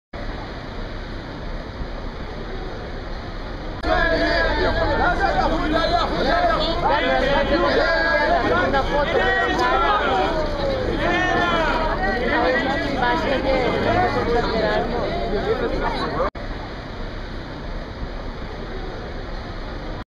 Ellen Von UNWERTH @ Paris Fashion Week September 2013 Show Gaultier
Ellen Von UNWERTH @ Paris Fashion Week September 2013 show défilé Jean Paul Gaultier - 28 septembre 2013